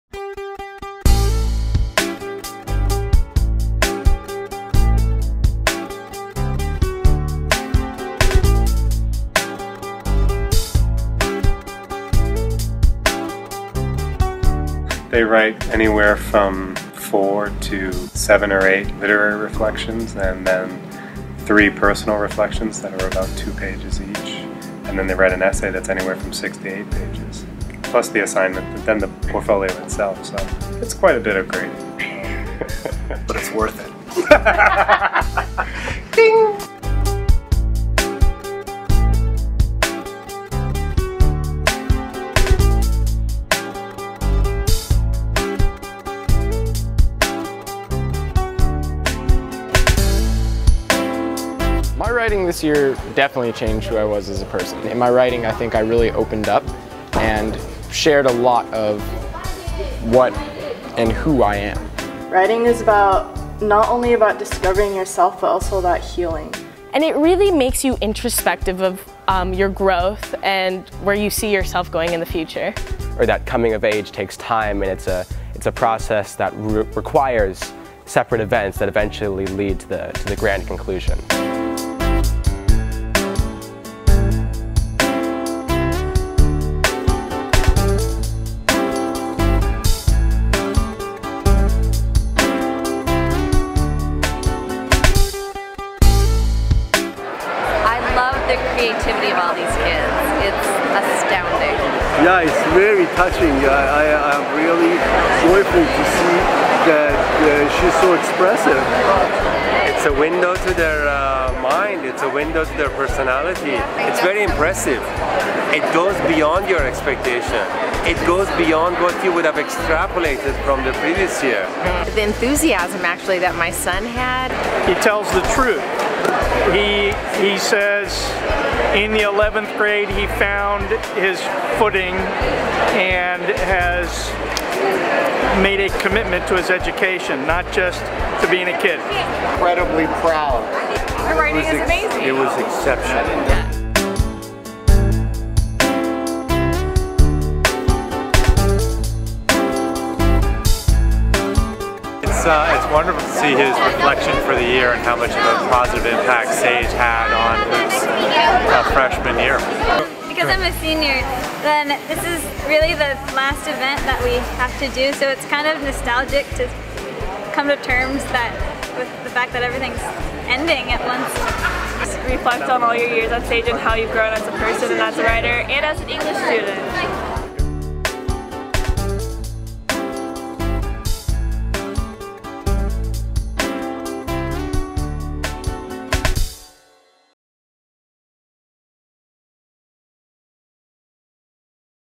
Sage Hill Portfolio Night
The tradition continues, as each Sage Hill student creates an English portfolio to reflect on his or her reading and writing for the year.

We come together as a community to share this work with teachers, classmates and families.